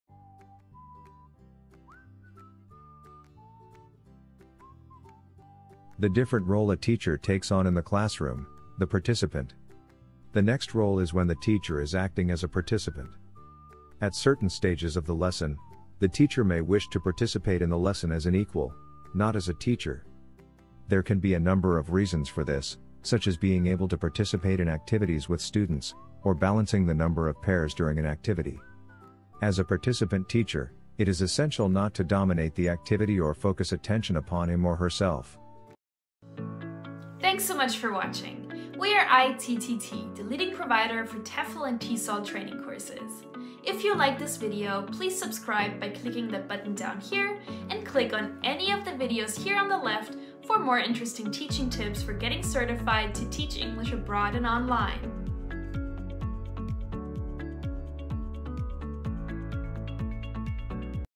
Roles A Teacher Takes On In The ESL Classroom - The Participant
Are you an ESL teacher and want to build on a successful teaching career? Watch this video to learn more about your main functions in the classroom. It's crucial for succeeding as an ESL professional.